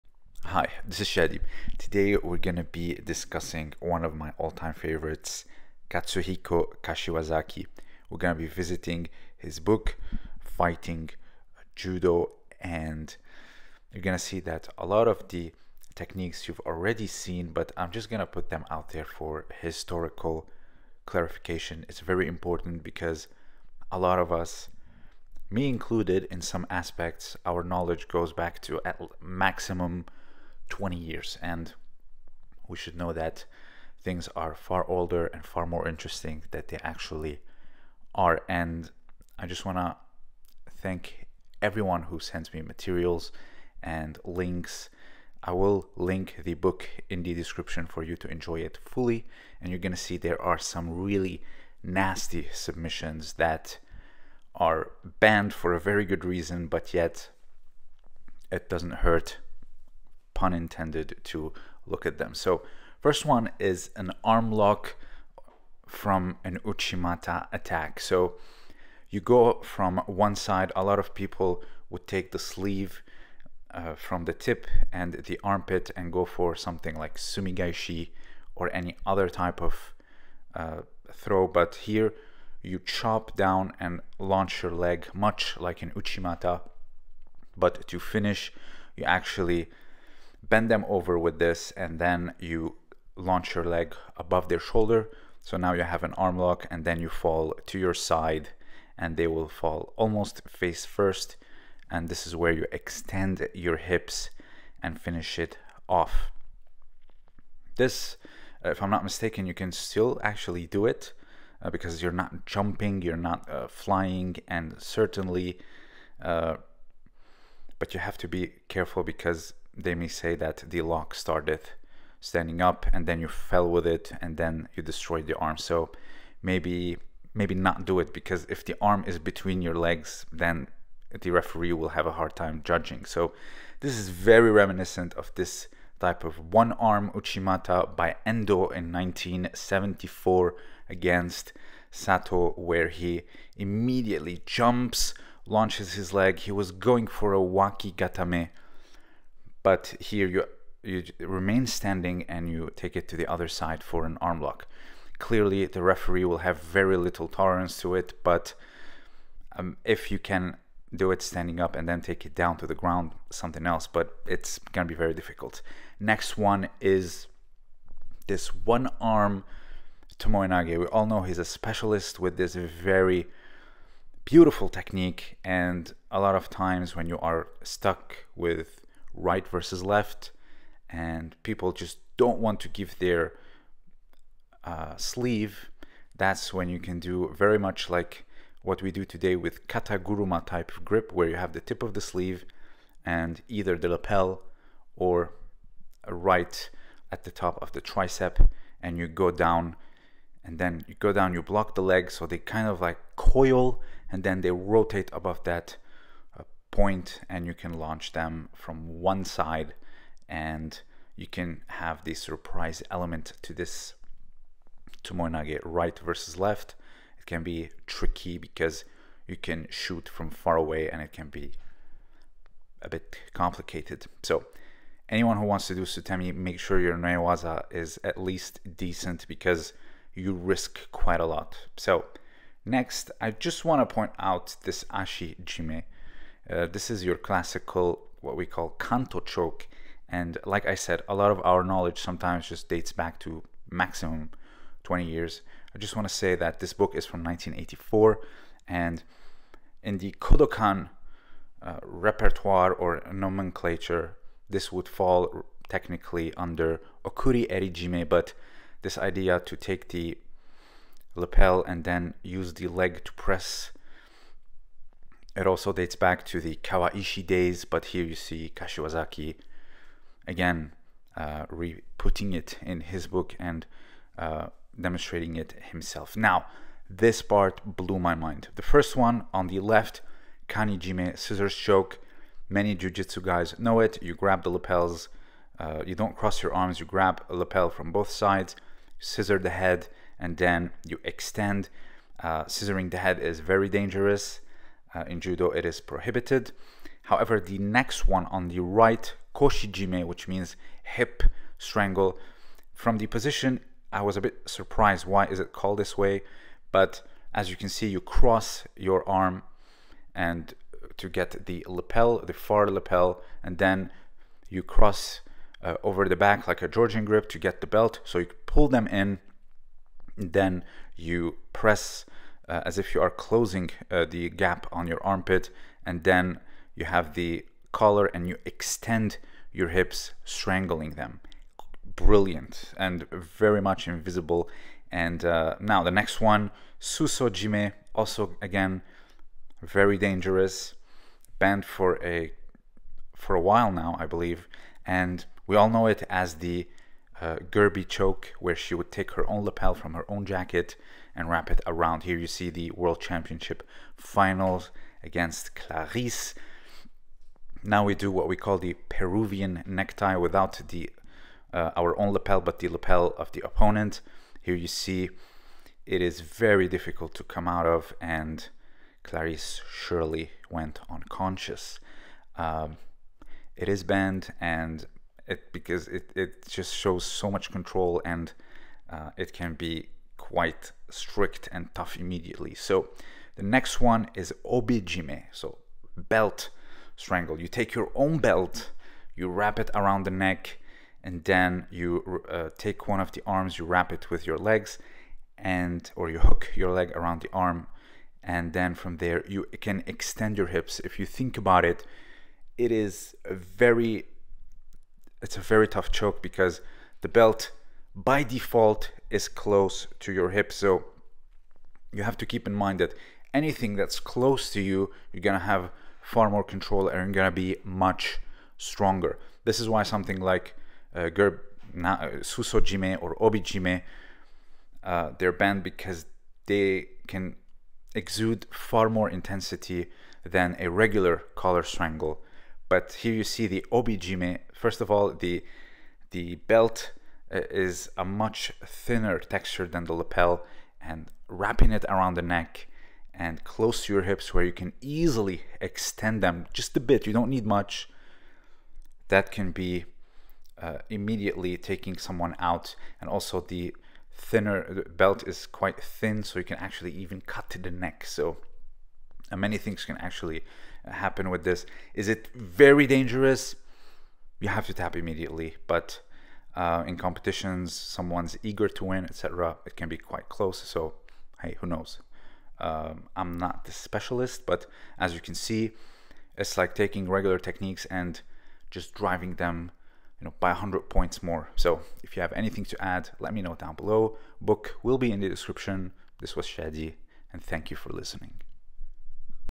Kashiwazaki reveals BANNED & LETHAL submissions
This video discusses the intricate, and wonderful style of Judo, the Judo of Kashiwazaki and dangerous submissions from his book.

My book "Une silhouette de mots":